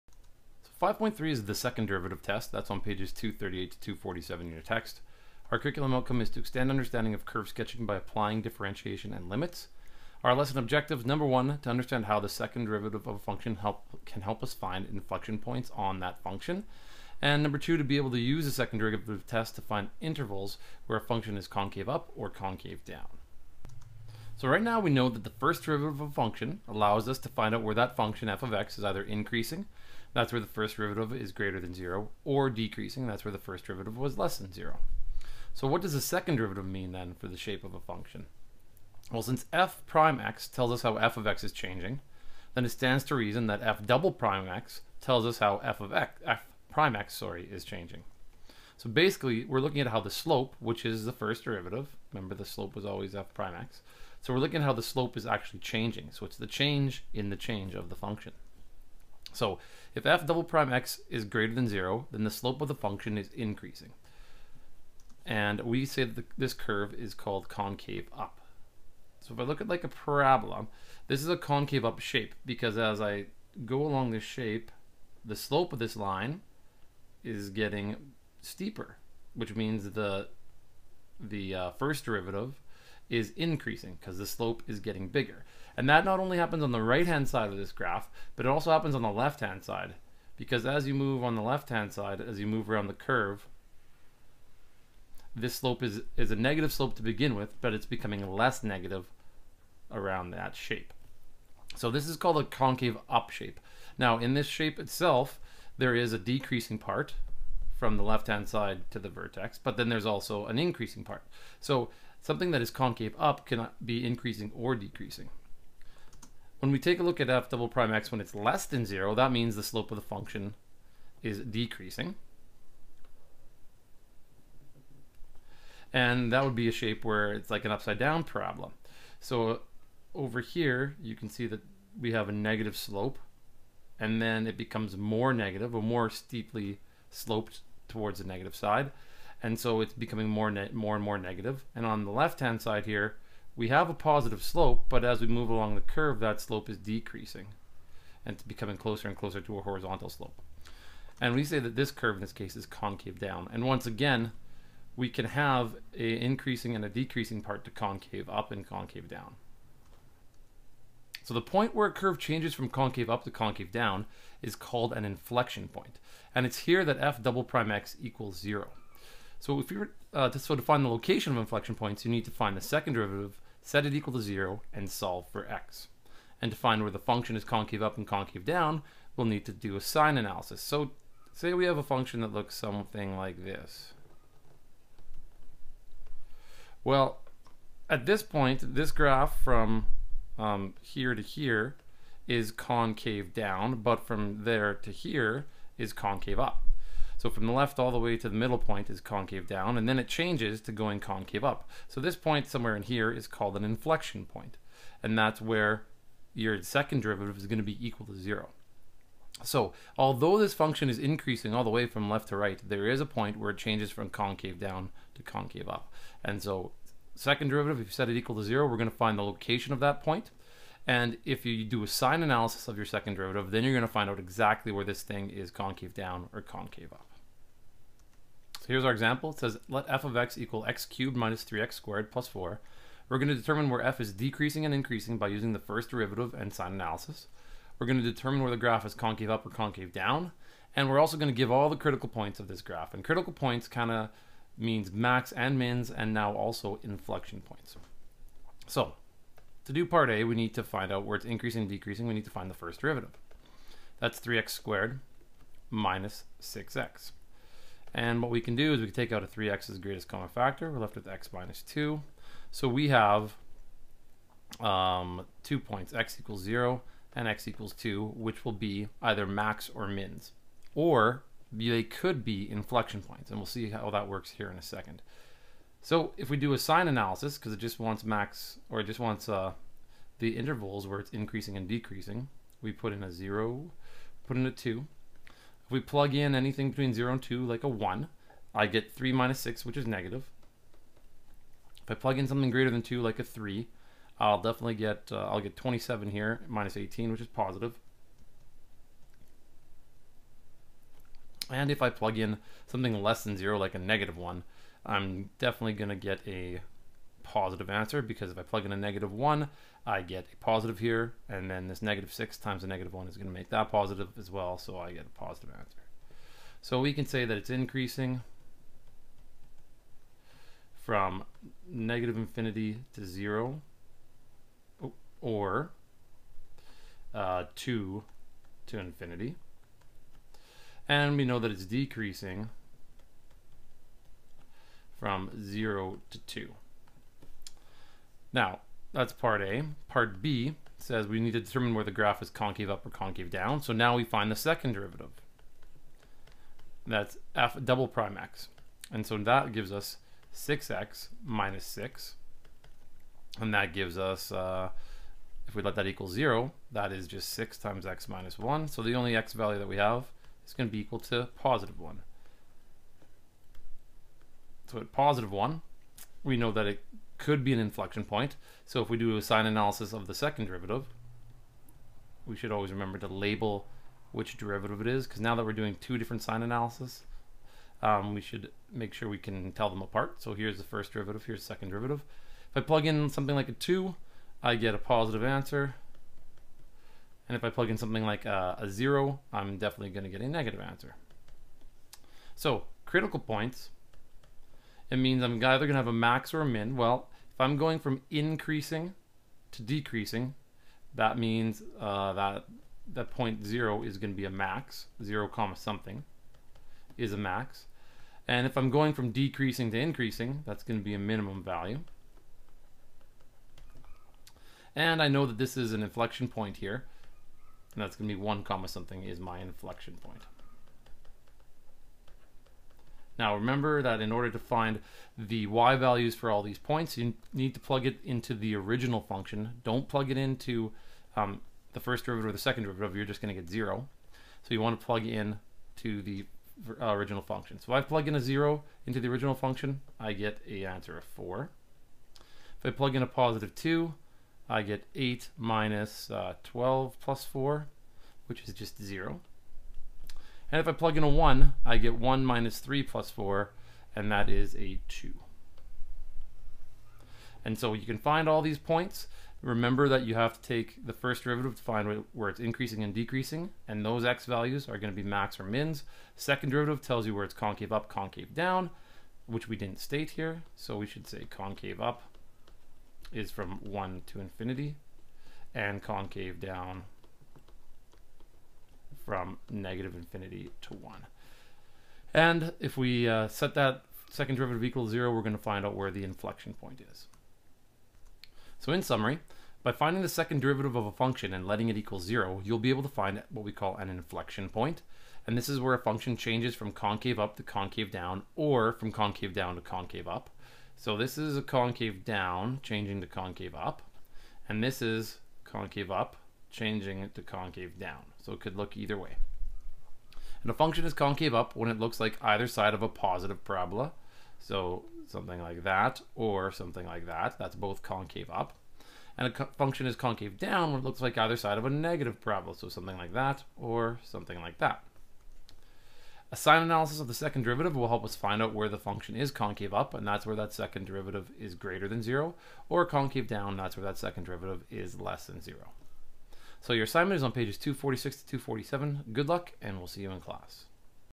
5.3 The Second Derivative Test
A lesson about finding inflection points and concavity by examining the second derivative of a function.